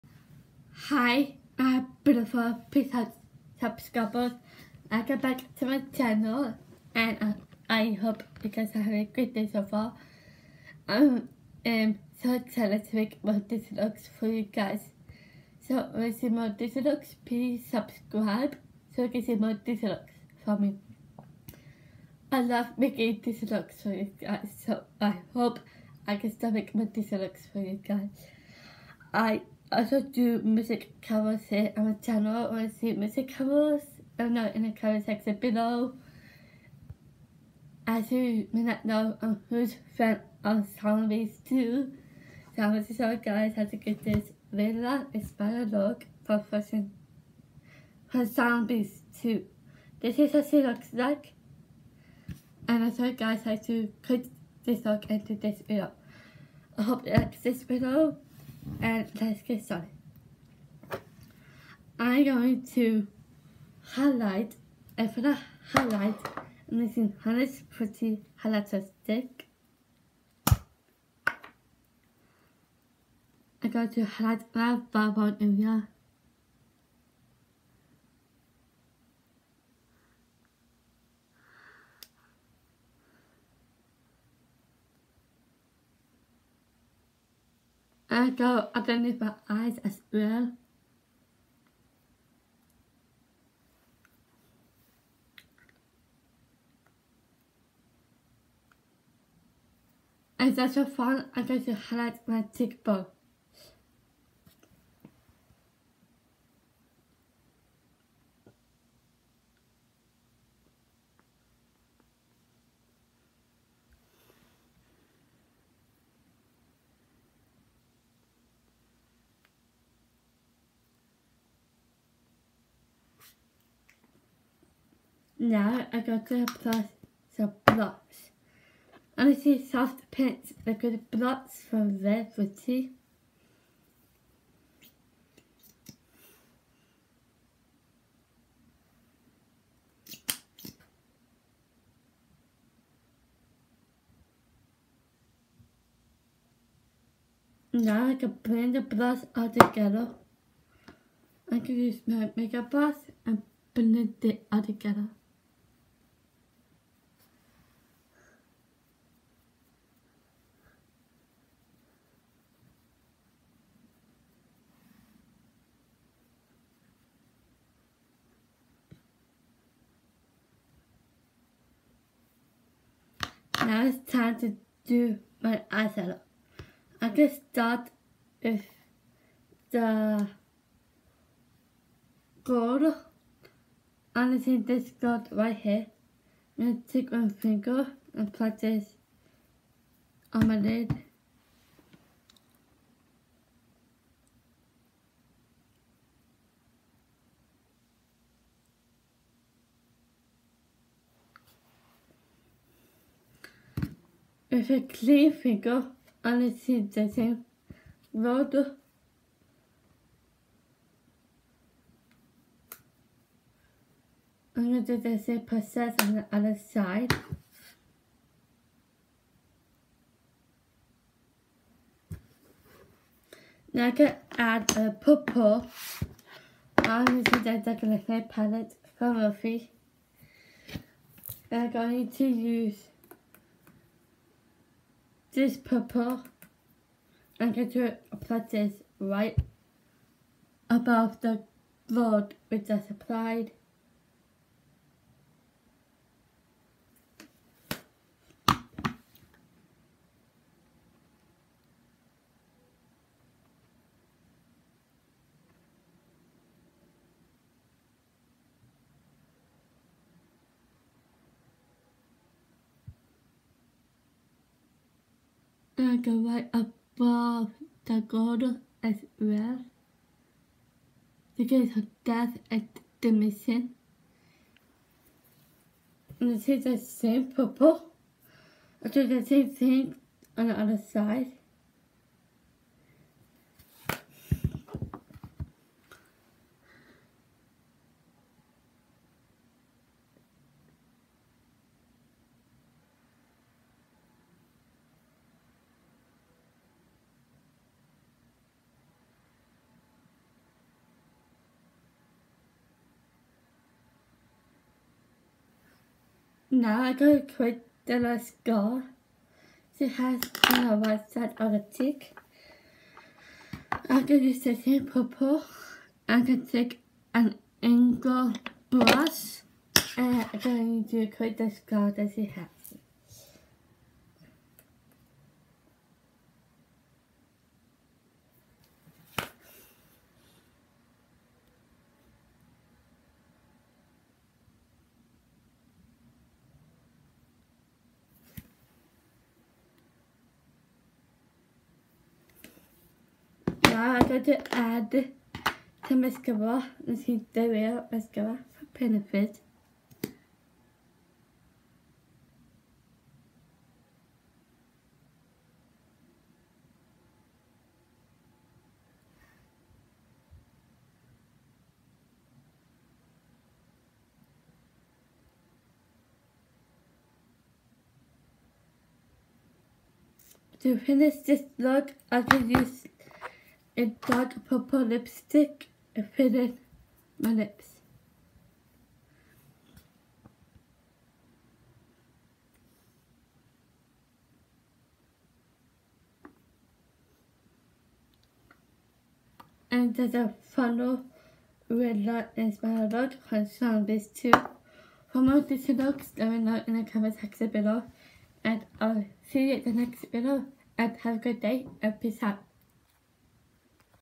Zombies 2 Willa makeup look
hope you guys are having a great day so far as you may not know i’m am
a huge fan of the show Zombies 2 so i wanted to show you guys how to
get this Willa inspired look from zombies 2

Honest beauty highlighter stick
Rare beauty soft pitch liquid blush
Rare beauty treat myself eyeshadow platte
Jacklyn Hill MORPHE eyeshadow platte
Benefit they’re real mascara
Wildfire dark purple lipstick

you guys can watch Zombies 2 on Disney Plus
Z-O-M-B-I-E-S 2
Zed and Addison continue to steer Seabrook High toward unity! But the arrival of a group of mysterious werewolves shakes things up

thank you for your love and support it helps me to make more amazing
Disney inspired looks for my beautiful peace out subscribers for more